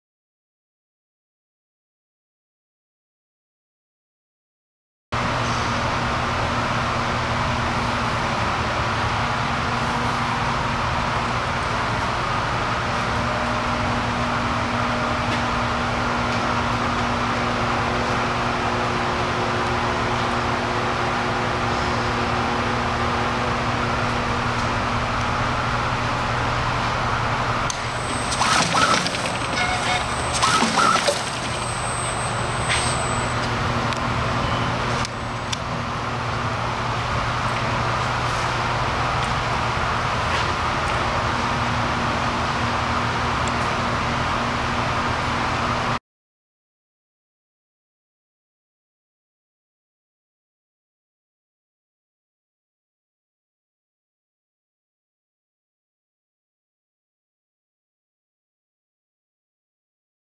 E315049 - UltraSource Posi 200 In Line Top and Bottom Labeler
This is a functional video of E315049 - UltraSource Posi 200 In Line Top and Bottom Labeler. for more available equipment.